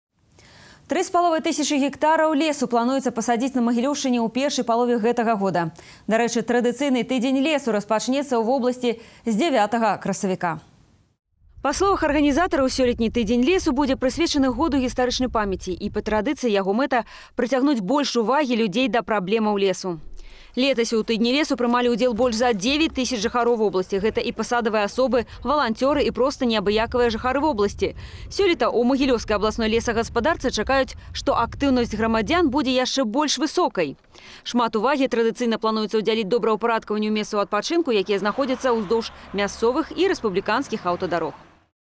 Неделя леса стартует в Могилевской области
Традиционная Неделя леса стартует в Могилевской области 9 апреля. Планируется массовая посадка деревьев, благоустройство мест отдыха вдоль местных и республиканских автодорог. В прошлом году в акции приняли участие более 9 тысяч жителей Могилевщины.